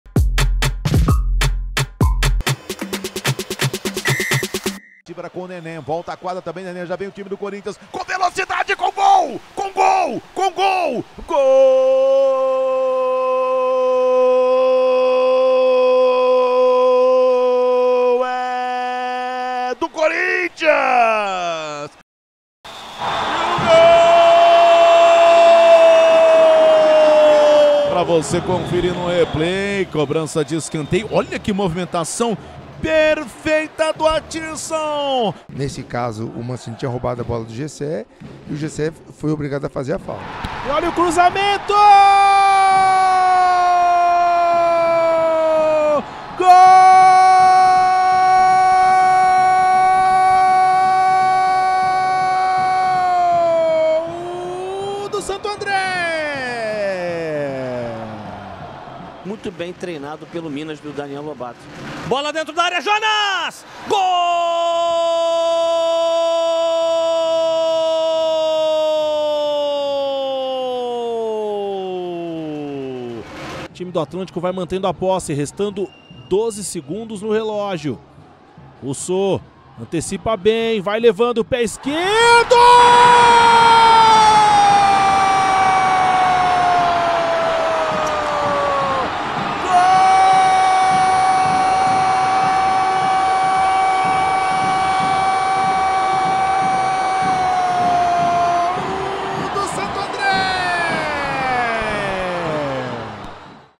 TOP 5 ASSISTÊNCIAS | OITAVAS - IDA | LNF 2024
Nossa equipe selecionou as TOP 5 assistências dos jogos de ida das oitavas de final da LNF 2024!

Curtiu? Faltou alguma? Comenta aí!

📽️LNFTV